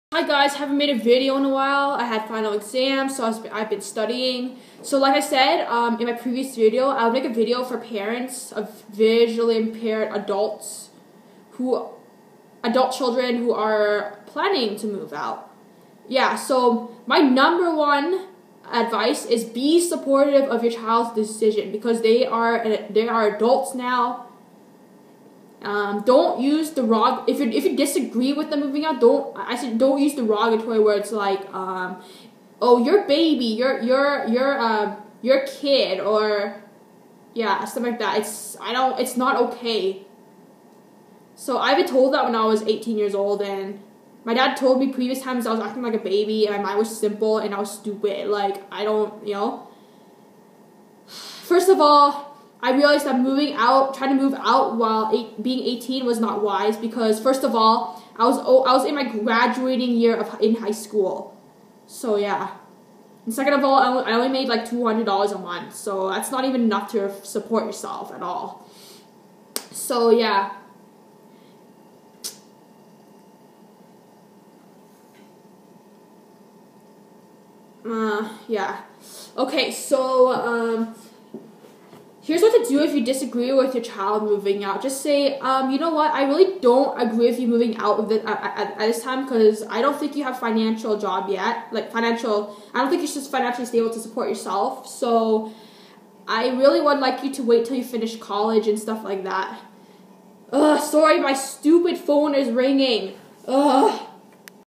Advice for parents of Visually Impaired Adult Children
Tis is the firs tart of my video. I am making this video to help parents who have visually impaired adult children who are moving out.